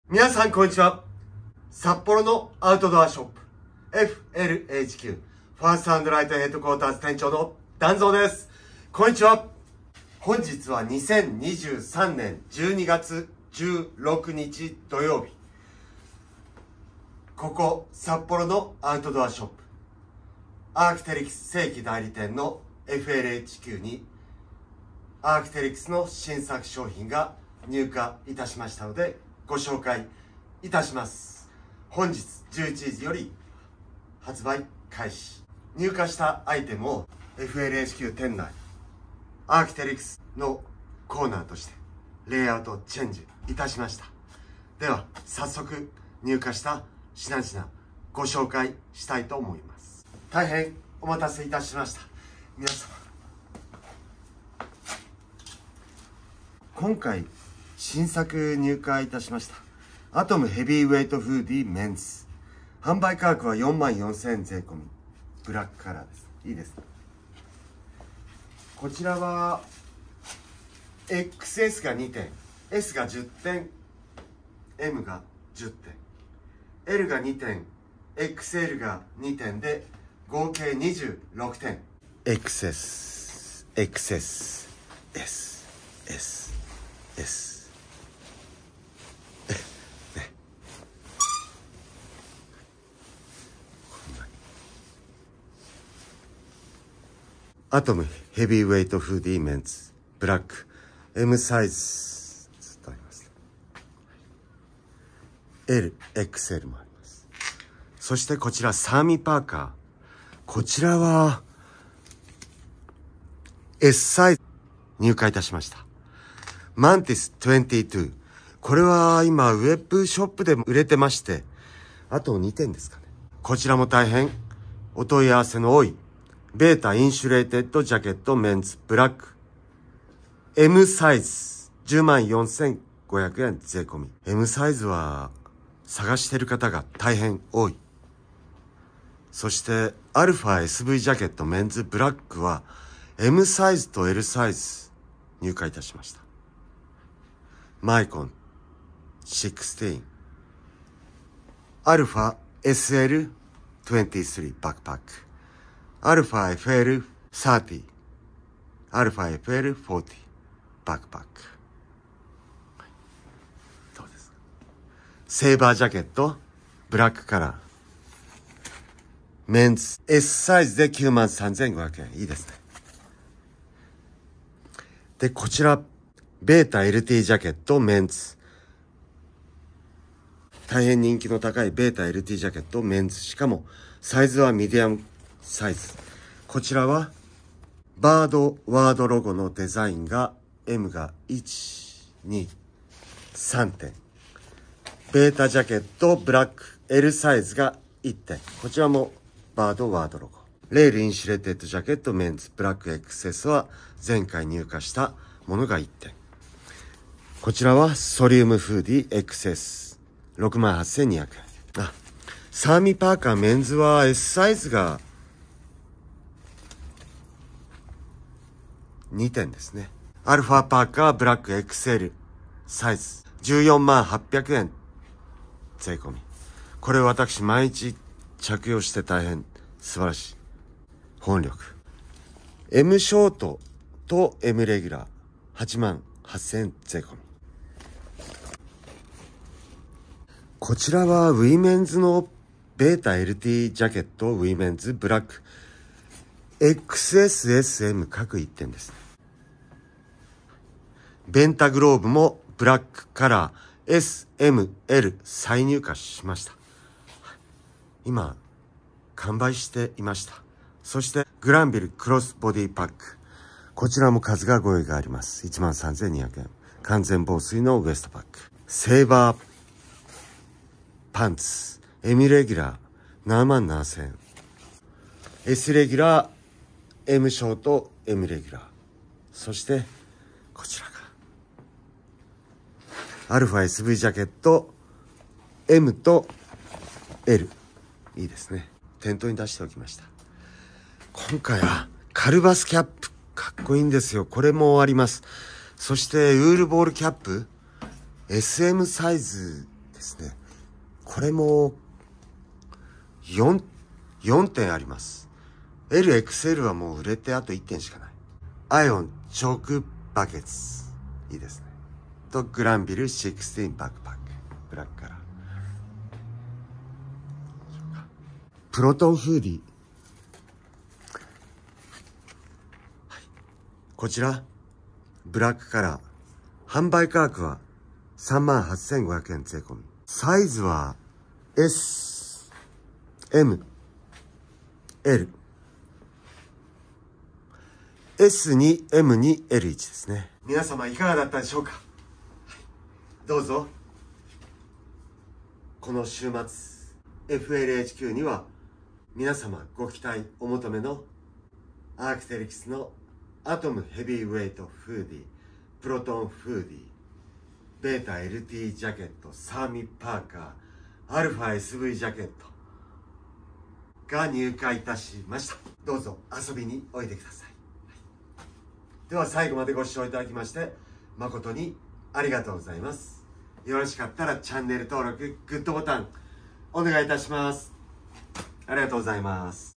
【ARC'TERYX 】最新入荷情報2023年12月16日(土)【ATOM HEAVYWEIGHT HOODY men's】
みなさん、こんにちは。本日は2023年12月16日土曜日。今回動画は、ARC'TERYX 正規代理店、札幌のアウトドアショップFLHQの最新入荷情報をご紹介します。